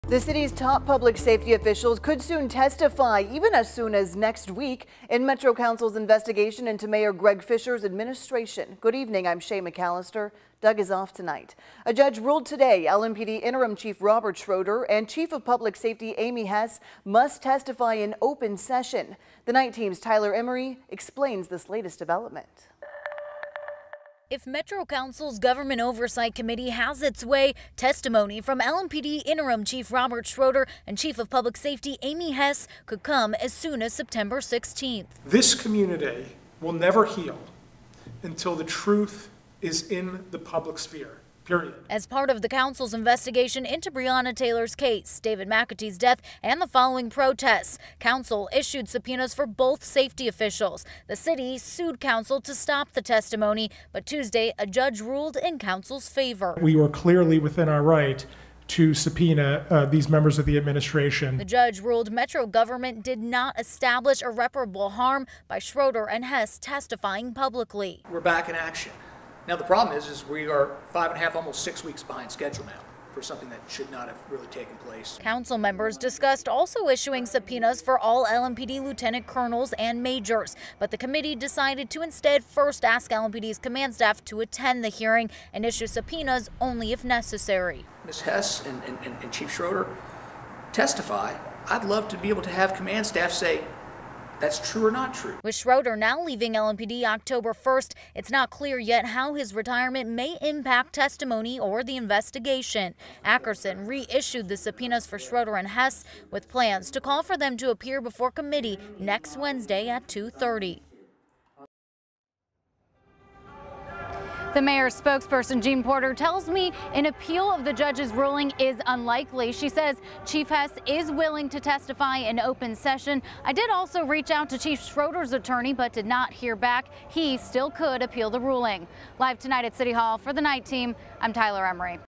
LMPD, public safety chiefs must testify before committee, judge rules
The city's top public safety officials could testify in Metro Council's investigation into Mayor Greg Fischer's administration.